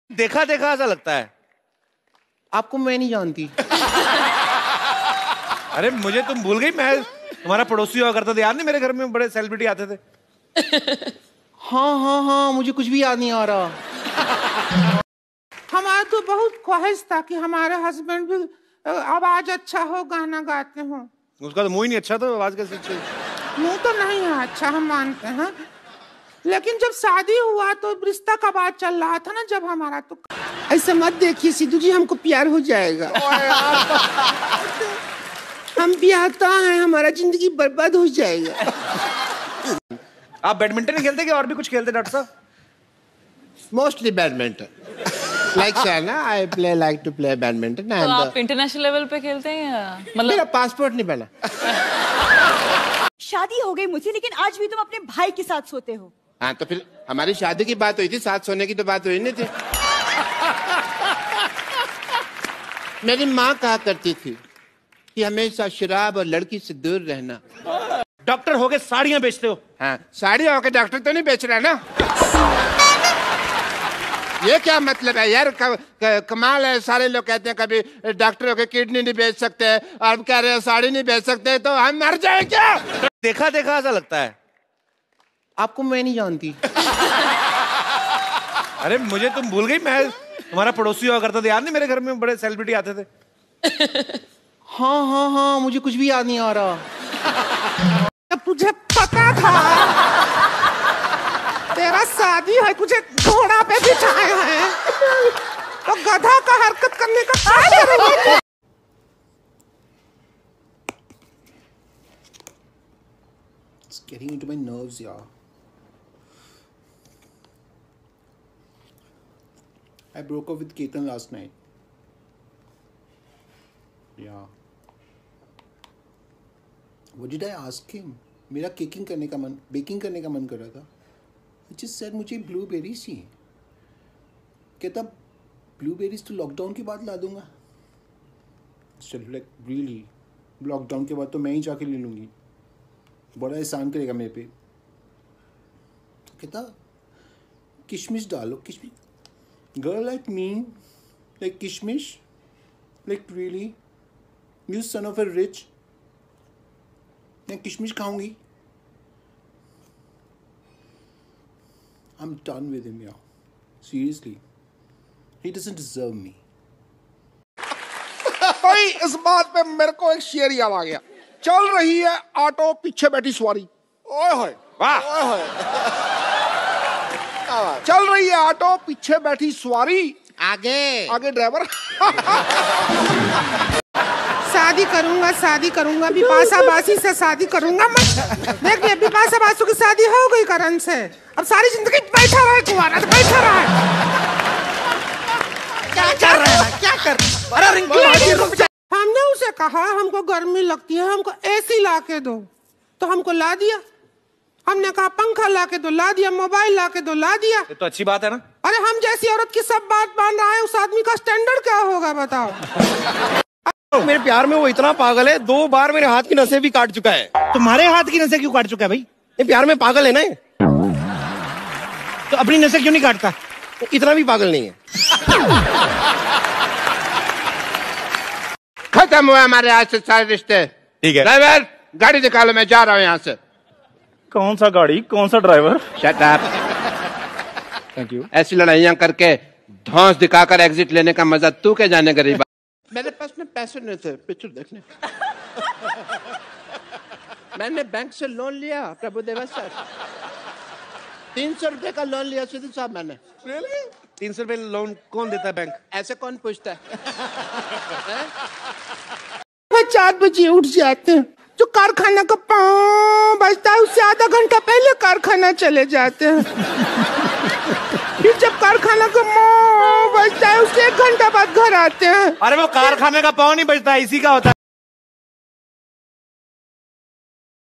Sunil Grover Best Funny(comedy_video) Compilations, easy time spending video Best 2020
Tiktokers act on Indian comedy king Sunil Grover Funny dialogues.